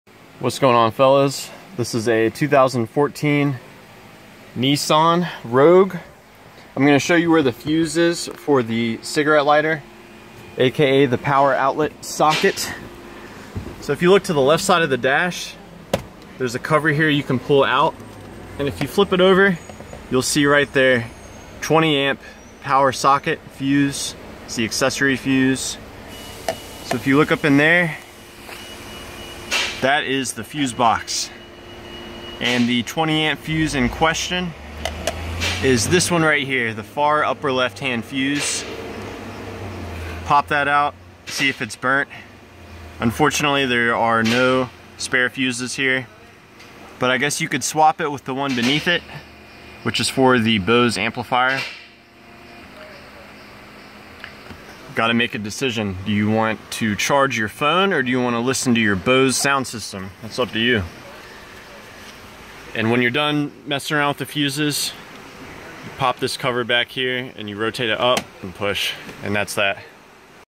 2013* Nissan Rogue Power Outlet/Cigarette Lighter Fuse
Using a 2013 Nissan Rouge I show you the location of the 20 amp fuse for the cigarette lighter aka power outlet